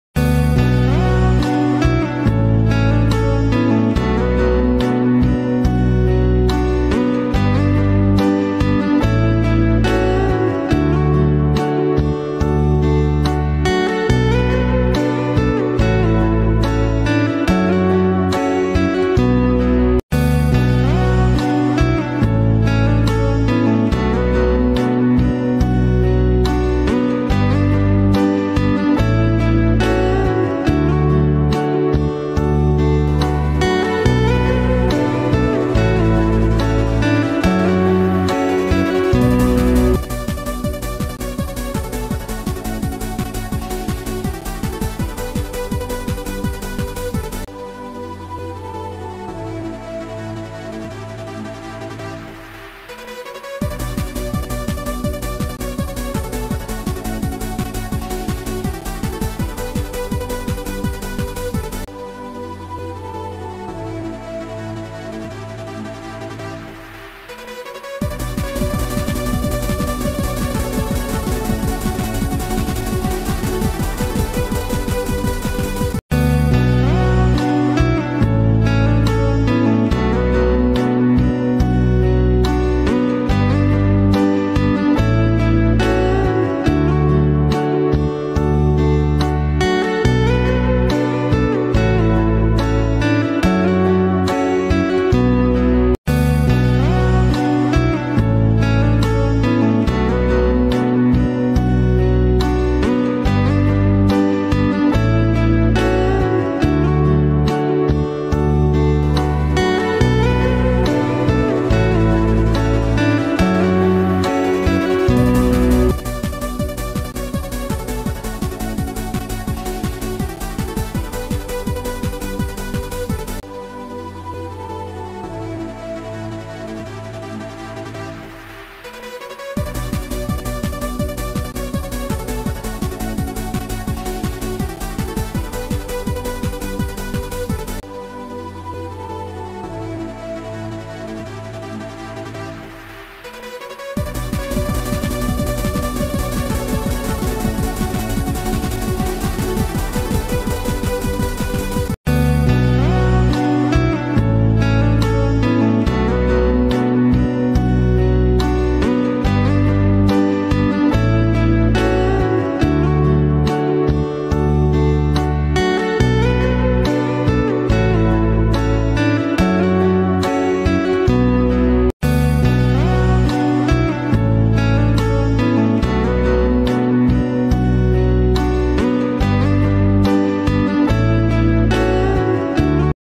Natural Beauty Extraordinary Natural Landscapes of the World Watch and Experience
In this video, we explore the extraordinary natural landscapes of our planet. From majestic mountains to the immense power of the oceans, and the uninterrupted life of wild natural creatures, this video allows you to witness the unseen beauty of Earth and marvel at its splendor.